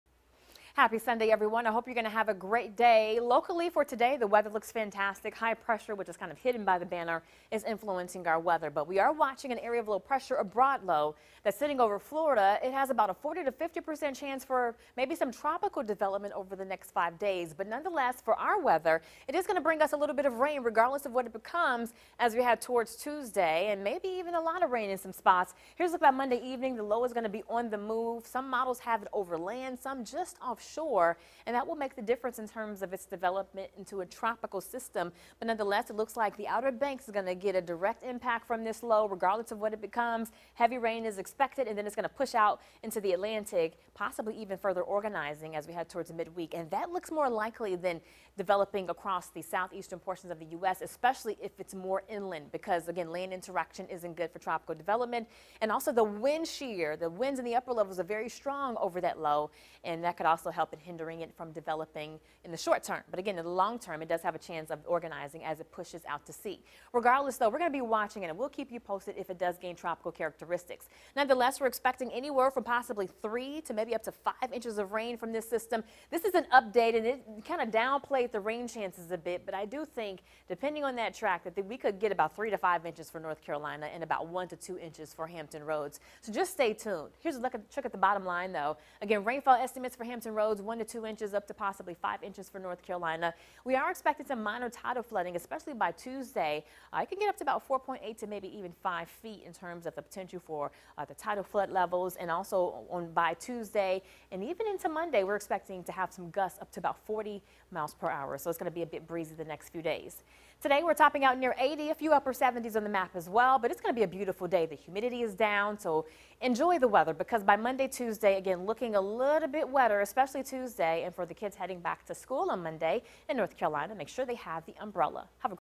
Sunday AM 8/27 Super Doppler 10 Forecast
Sunday AM Forecast with Meteorologist Deitra Tate